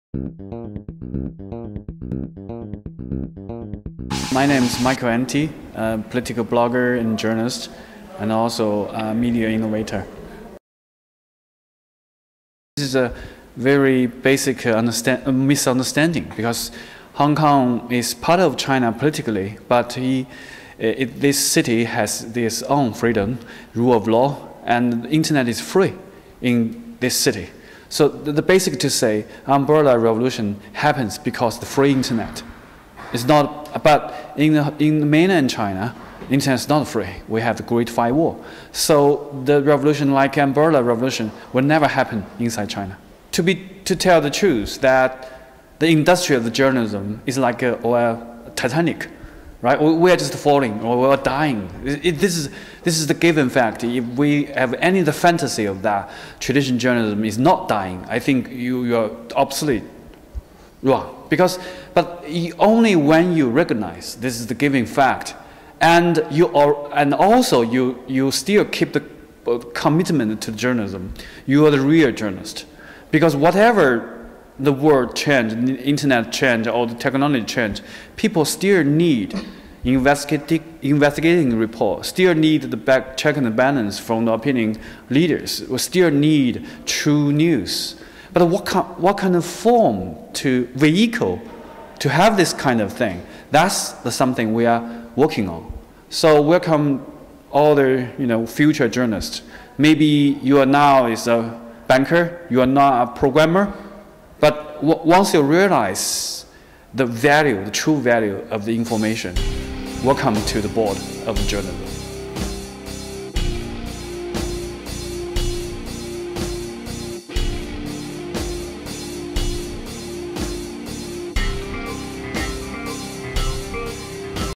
Michael Anti: "el periodismo tradicional no está agonizante, está obsoleto"
Jing Zhao, más conocido por su seudónimo Michael Anti, (@mranti), es un destacado periodista chino, blogger político, innovador en medios y una figura clave en el nuevo periodismo en China.
Michael Anti  nos habla de la Umbrella Revolution o la Revolución de los paraguas, así como de  la situación del periodismo a nivel global.